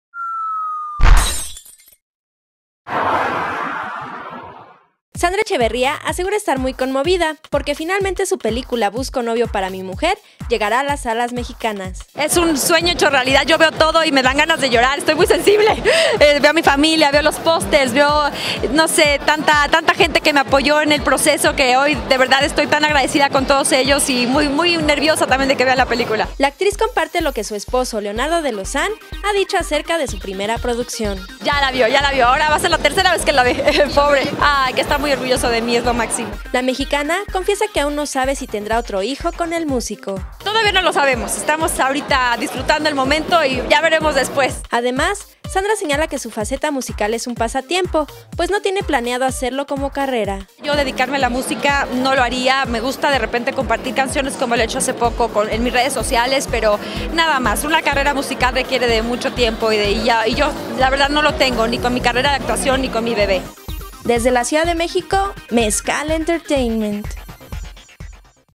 Sandra Echeverría: " Leonardo está muy orgulloso de mí"
La actriz presenta "Busco Novio Para Mi Mujer", primera película bajo su producción, y su esposo le dijo que está muy orgulloso de ella por haber materializado uno de sus sueños.

¡Visítanos y encuentra tu dosis diaria de entrevistas exclusivas, últimas noticias y chismes de las celebridades!